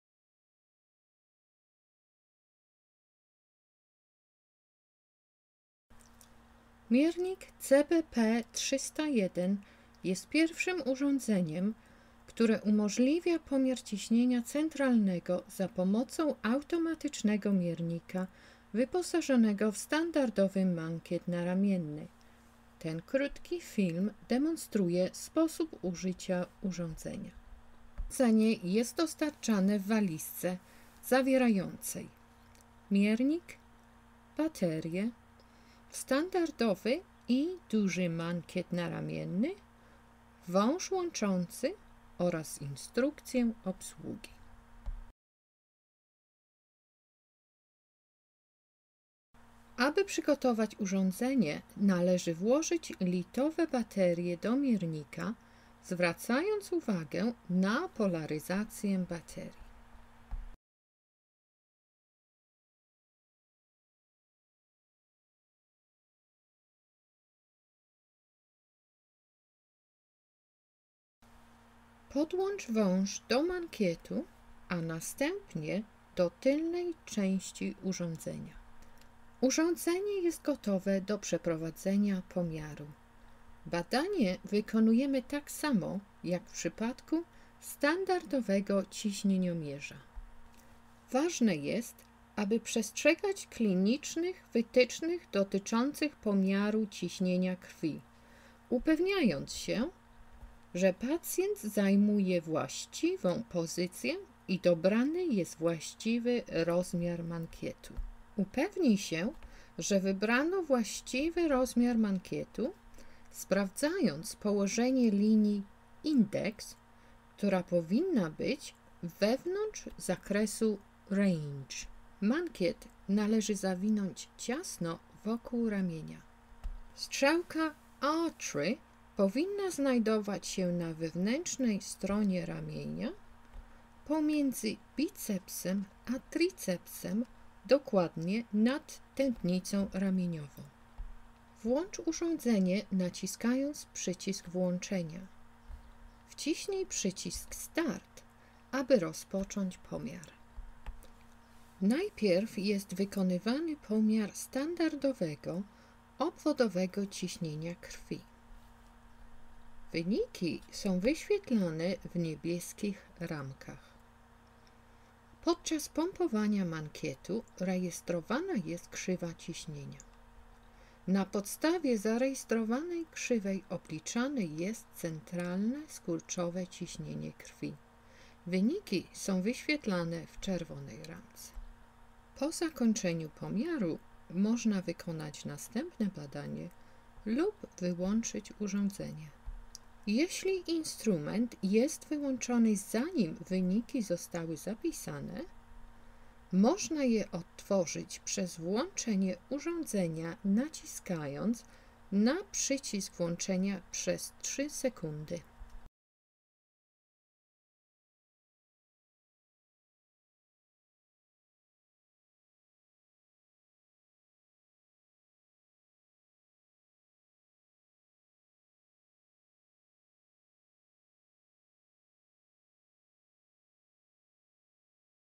Miernik centralnego ciśnienia krwi cBP301
Parametry:
- technologia opracowana w King's College London
- precyzyjny pomiar ciśnienia centralnego z mankietu naramiennego
- pomiar ciśnienia obwodowego i centralnego
- metoda szybka i nieinwazyjna
- miernik bardzo prosty w obsłudze